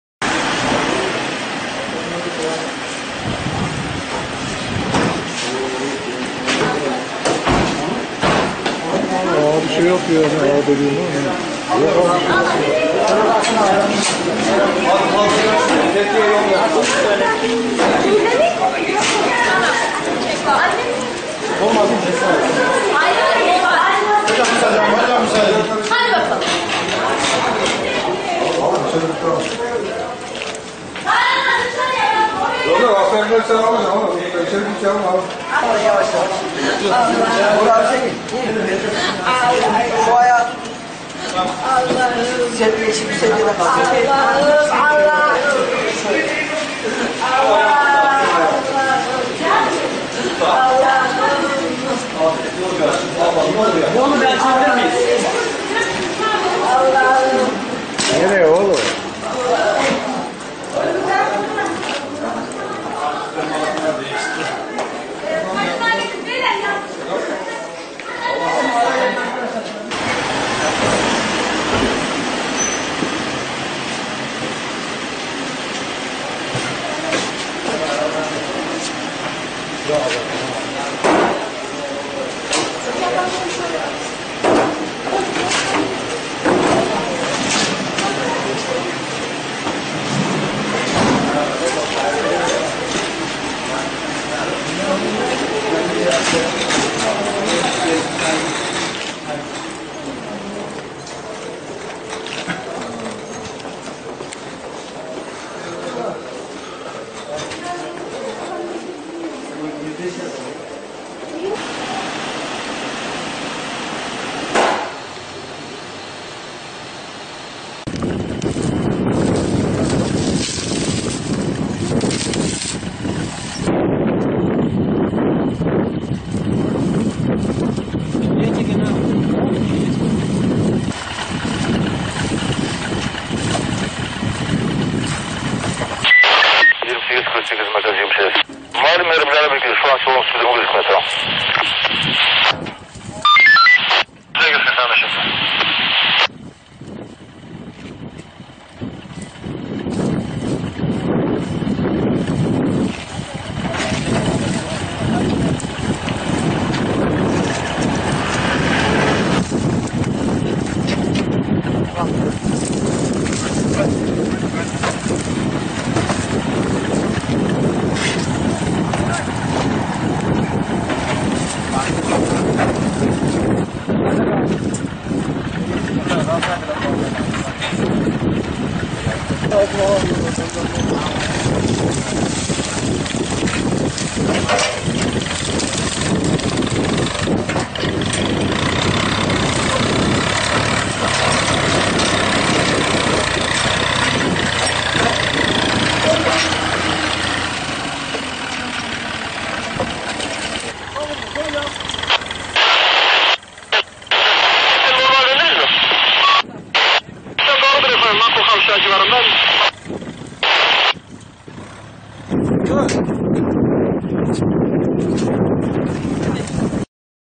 Otomobil Koyun Sürüsüne Daldı 3 Yaralı, 45 Koyun Telef Oldu
Karaman'da direksiyon hakimiyetini kaybeden sürücü yolun karşısına geçmekte olan koyun sürüsünün arasına daldı. Kazada 45 koyun telef oldu.